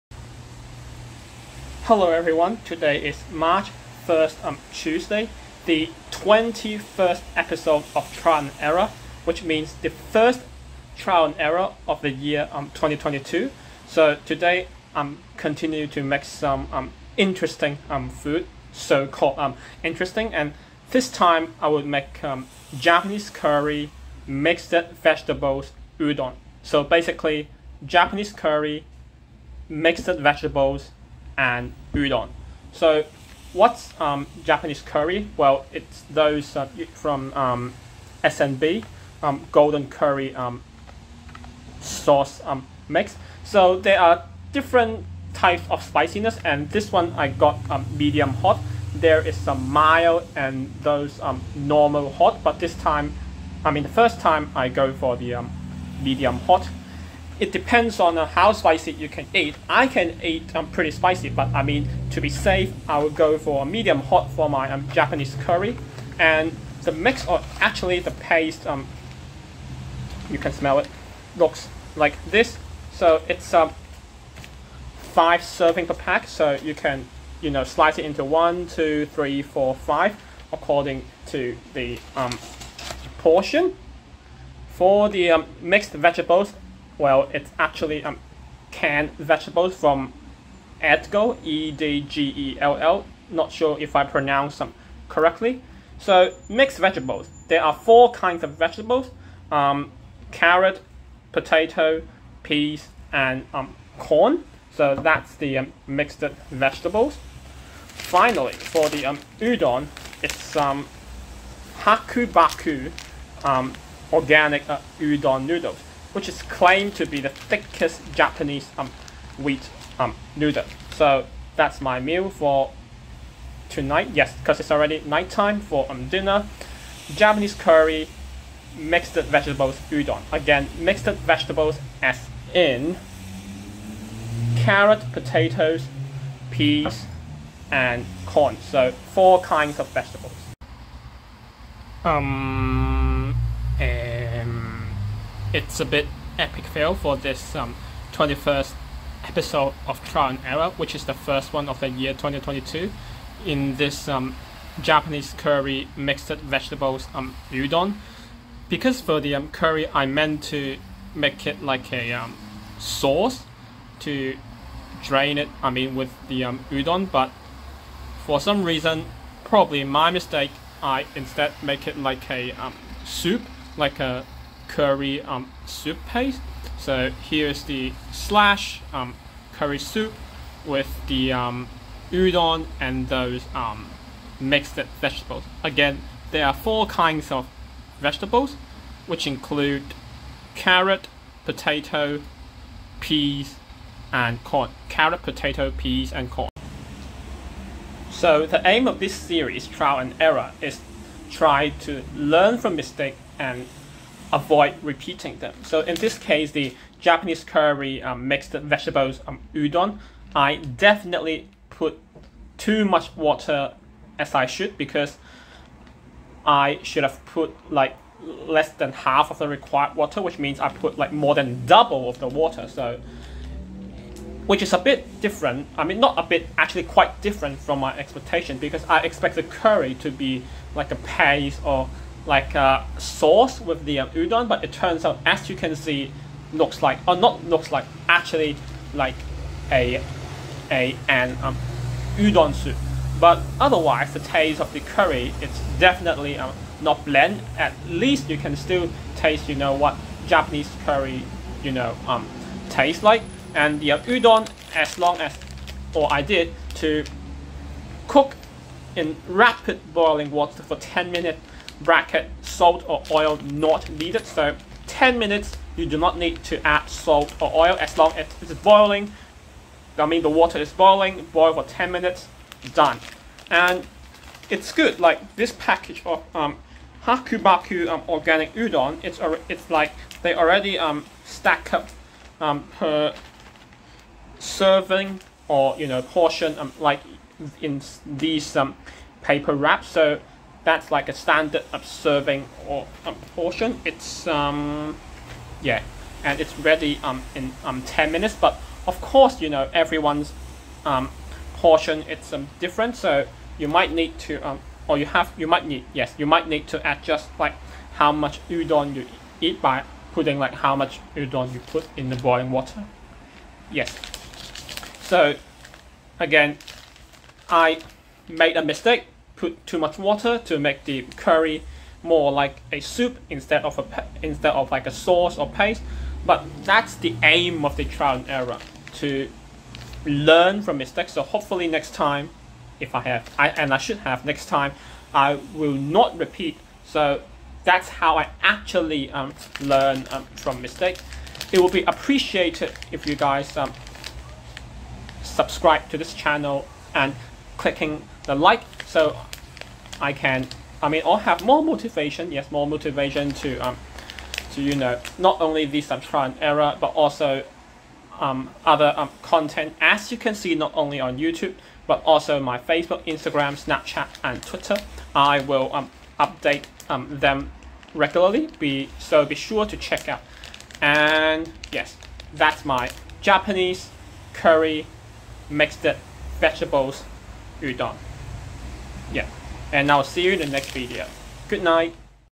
isaactrialanderror 21.0 馮場作試當真21.0
Japanese Curry Mixed Vegetables Udon
AUD 2.6 (correct to the nearest 0.05)
The Vegetables are Carrot, Potato, Peas and Corn

Supermarket: Coles Epping
Address: 53/57 Rawson St, Epping NSW 2121